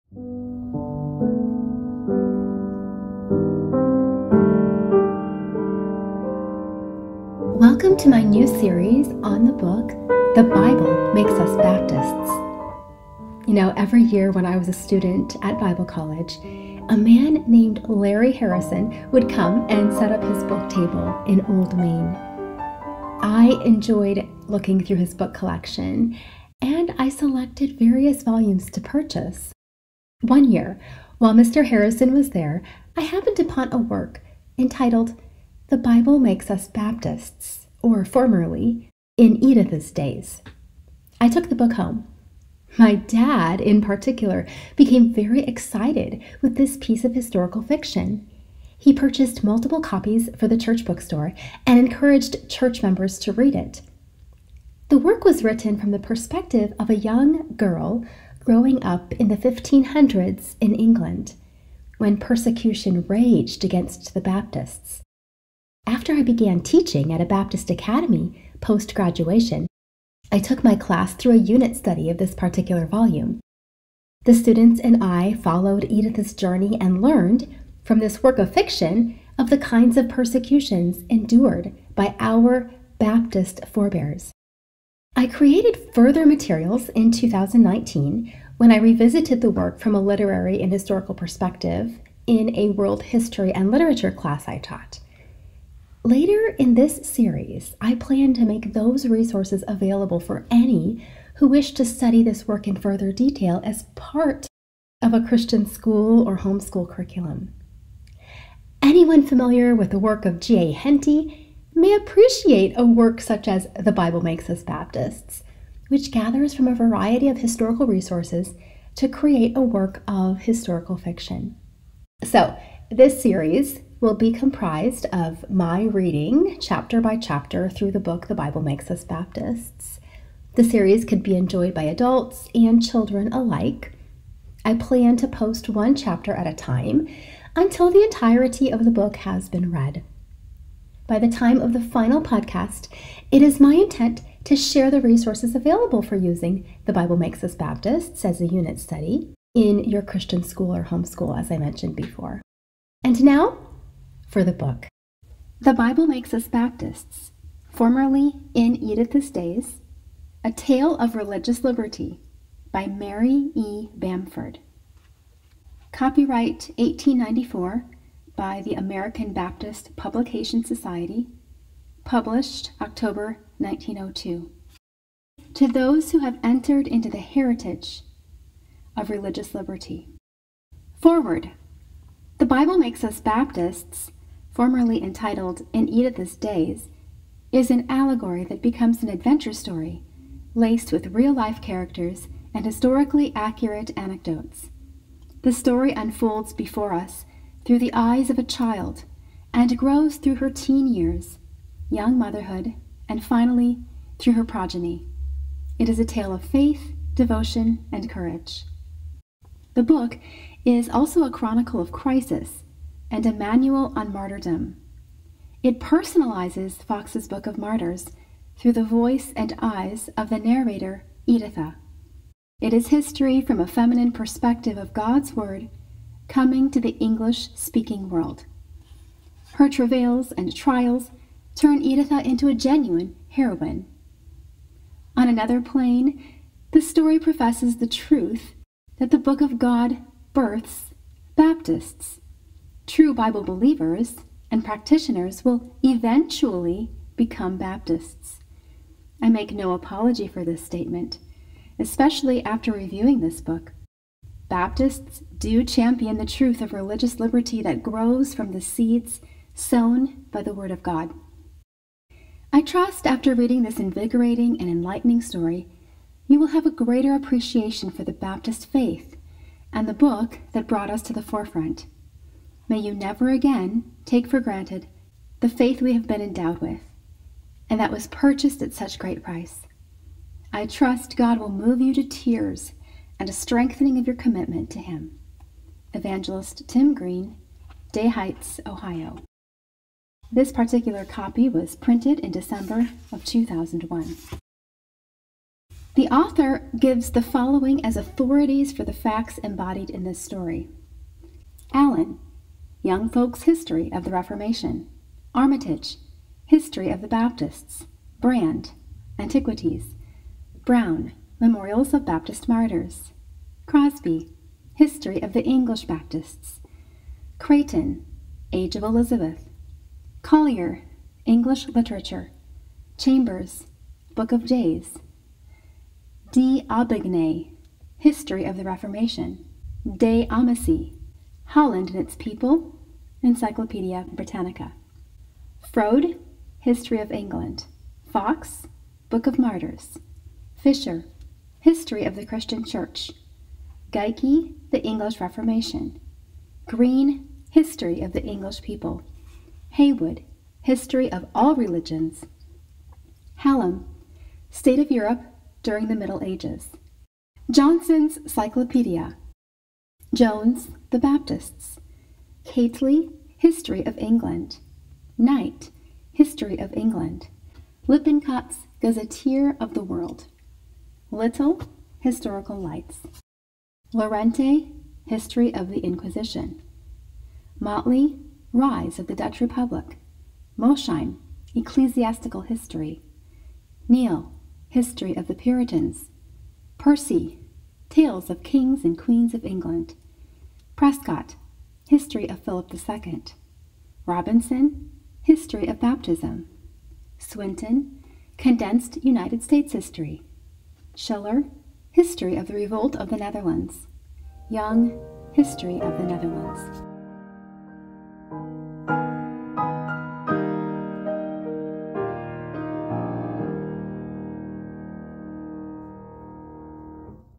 The Bible Makes Us Baptists, Introduction (Tethered to Truth, for Christian Ladies by Heather Ross)
Every year when I was a student at Bible college, a man named Larry Harrison would come and set up his book table in Old Main.  I enjoyed looking through his book collection and selecting various volumes to purchase. One year while Mr. Harrison was there, I happened upon a small book entitled, The Bible Makes Us Baptists or formerly, In Editha’s Days.

I took the book home. My dad, in particular, became very excited with this piece of historical fiction. He purchased multiple copies for the church book store and encouraged church members to read it. The work was written from the perspective of a young girl growing up in the 1500s in England, when persecution raged against Baptists. After I began teaching at a Baptist academy, I took my class through a unit study of this particular volume. The students and I followed Editha’s journey and learned, from this work of fiction, of the kinds of persecutions endured by our Baptist forebears.

I created further materials in 2019 when I revisited the work from a literary and historical perspective in a world history and literature class I taught. Later in this series, I plan to make those resources available for any who wish to study this work in further detail as part of a Christian school or home school curriculum.

Anyone familiar with the work of G.A. Henty may appreciate a work such as this, which gathers from a variety of historical resources to create a work of historical fiction.

This series will be comprised of reading, chapter by chapter, through the book The Bible Makes Us Baptists. This series can be enjoyed by adults and children alike. I plan to post one chapter at a time until the entirety of the book has been read. By the time of the final podcast, it is my intent to share the resources available for using The Bible Makes Us Baptists as a unit study in your Christian school or home school.

And now, for the book . . .